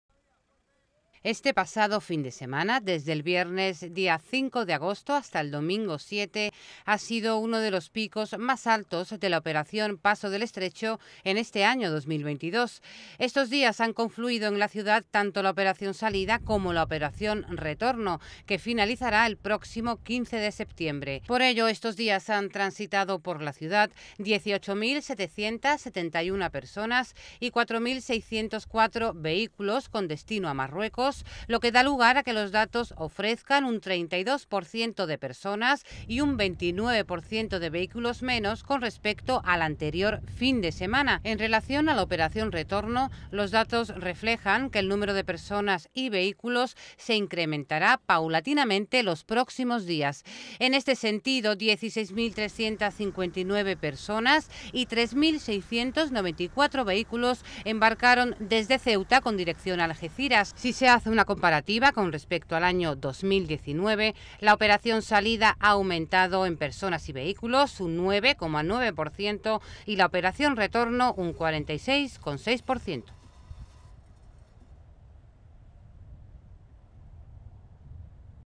Este pasado fin de semana han confluido tanto la Operación Salida, como la Operación Retorno
Este pasado fin de semana han confluido la Operación Salida, que ha contabilizado a más de 18.500 personas con destino a Marruecos, y la Operación Retorno, que ha dado lugar a que cerca de 16.360 personas hayan pasado por Ceuta con destino a la Península. Ambas, en el marco de la Operación Paso del Estrecho, que finaliza el 15 de septiembre